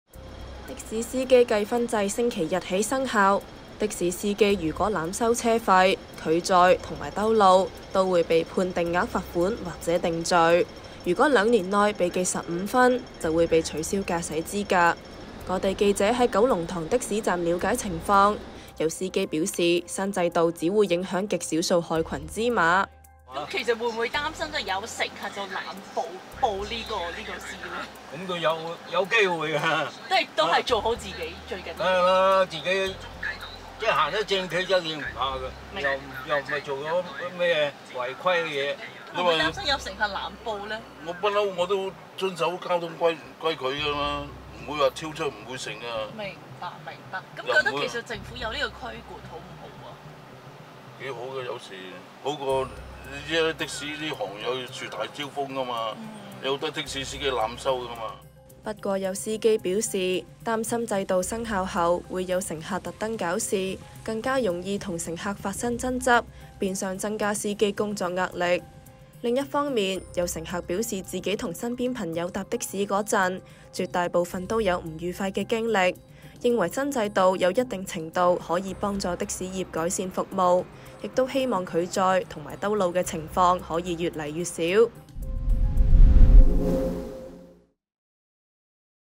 的士司機記分制︱有司機撐撥亂反正：僅影響極少數害群之馬 有司機憂「多爭拗增壓力」｜星島頭條新聞｜的士｜記分制｜生效｜新制度｜規管
的士司機記分制今日（22日）起生效，的士司機若濫收車費、拒載和兜路均會被判定額罰款或定罪，亦會被記最多10分，同時需自費修習並完成的士服務改進課程；若兩年內被記15分就會被取消駕駛資格。有的士司機和市民均認為新制能保障乘客，同時助行業「撥亂反正」，不過有的士司機則認為不合理，指措施實施後進一步增加工作壓力。